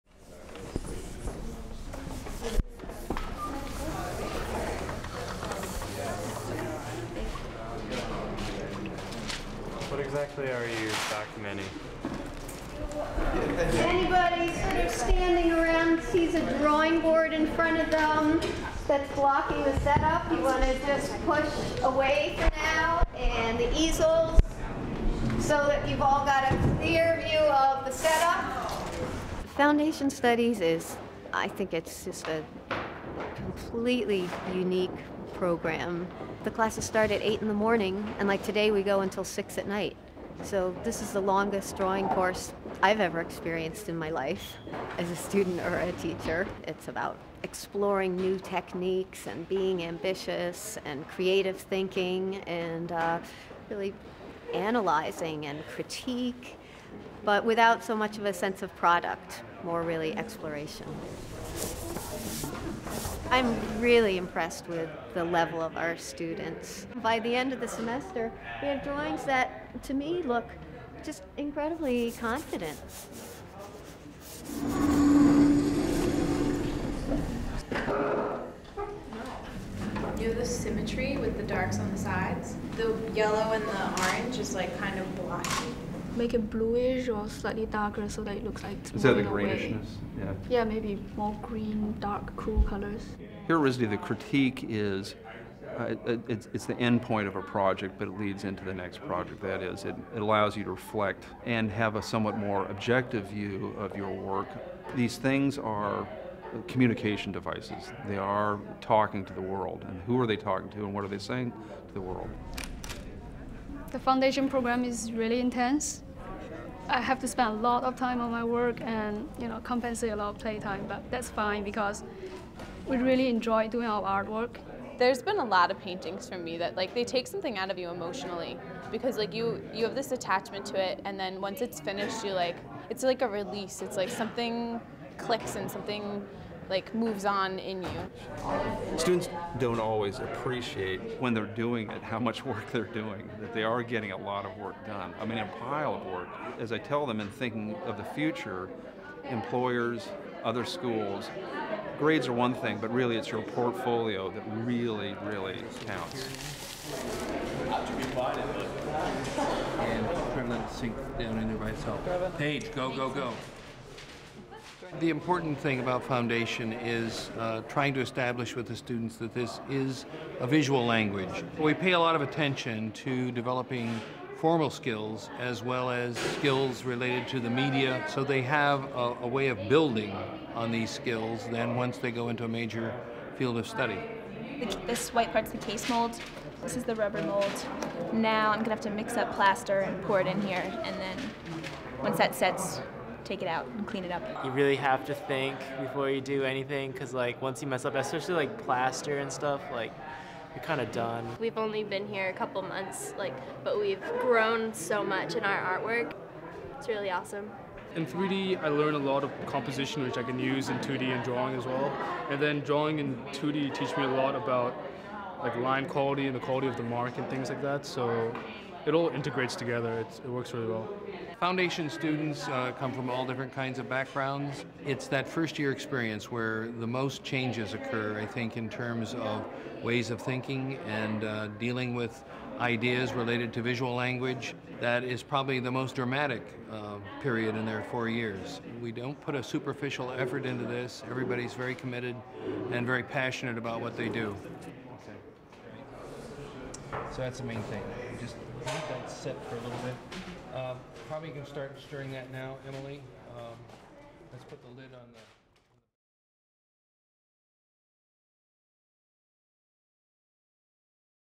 4 minutes in RISD's first-year studios
Tour Rhode Island School of Design's drawing, 2D and 3D foundation classrooms and see what it's like to be in the RISD freshman foundation program.